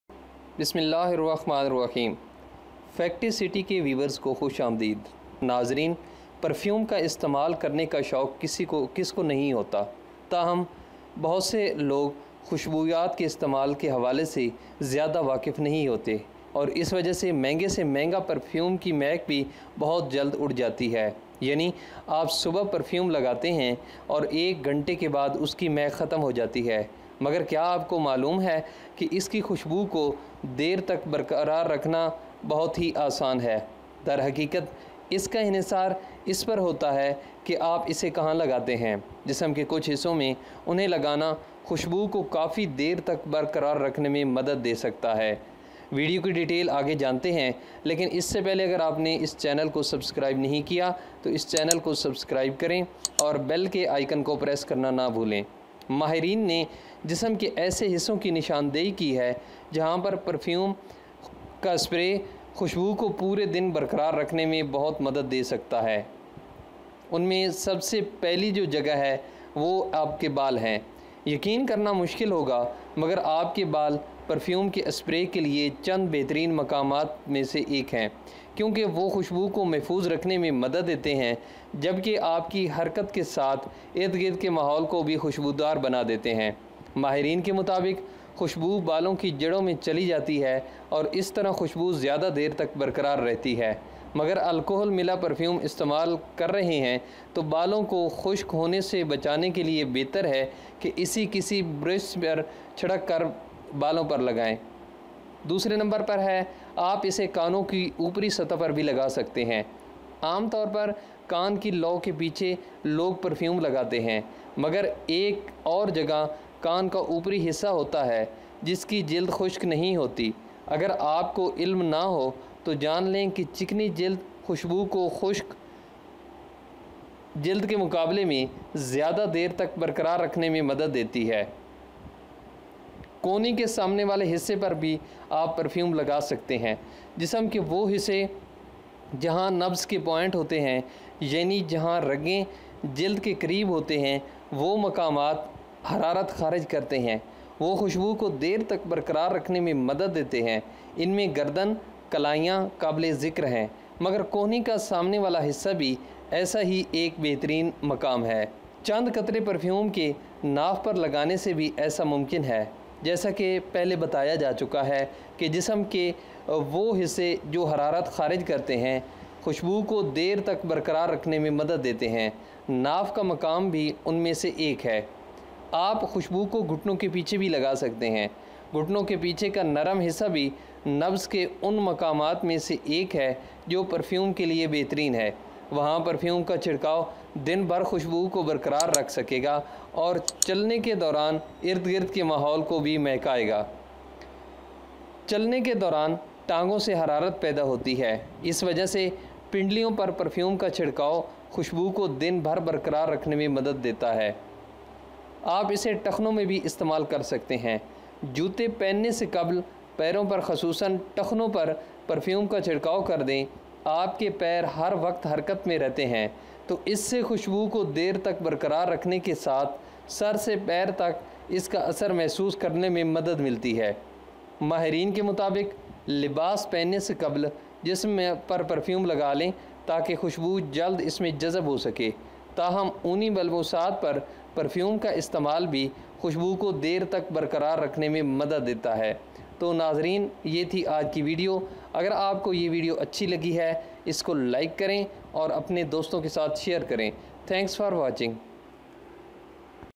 It is very Easy to Maintain the Fragrance of Perfume for a Long time
Who doesn't like to use perfume but most people are not very familiar with the use of perfumes and because of this the scent of the most expensive perfume disappears very quickly.

That is, you apply perfume in the morning and the scent disappears after an hour, but did you know that it is possible to maintain this scent for a long time?

In fact, depending on where you apply it, applying it to certain parts of the body can help retain the scent for a long time.

Experts have identified areas of the body where perfume sprays can help maintain the scent throughout the day.

In the hair
It's hard to believe, but there are some great places to spray your hair perfume, as they help preserve the scent while making the surroundings fragrant with your movement.

According to experts, the fragrance goes to the roots of the hair and thus the fragrance remains for a long time, but if you are using alcohol mixed perfume, it is better to spray it on the hair and apply it on the hair to prevent it from drying out.

On the upper surface of the ears
People usually apply perfume behind the earlobes, but there is another area on the upper part of the ear that does not dry out. If you don't know, know that oily skin helps to retain fragrance longer than dry skin.

In the front of the elbow
The parts of the body where the pulse points are, ie where the veins are close to the skin, they emit heat, they help to retain the scent for a long time, notably the neck and wrists, but the elbows. The front part is also a great place to be.

Navel
It is also possible to apply a few drops of perfume on the navel, as mentioned above, the parts of the body that emit heat, help to retain the scent for a long time, the location of the navel is one of them. ۔

Behind the knees
The soft part of the back of the knee is also one of the best places for perfume. Spraying perfume there will keep the scent intact throughout the day and will also make the surroundings smell while walking.

Under the shins
The legs generate heat while walking and therefore spraying perfume on the shins can help maintain the scent throughout the day.

In the ankles
Before wearing shoes, spray perfume on the feet, especially the ankles. Your feet are in motion all the time, so it helps to keep the scent for a long time and feel the effect from head to toe.

In clothes
According to experts, apply perfume on the body before wearing so that the fragrance can be absorbed into the skin, however, the use of perfume on woolen garments also helps to retain the fragrance for a long time.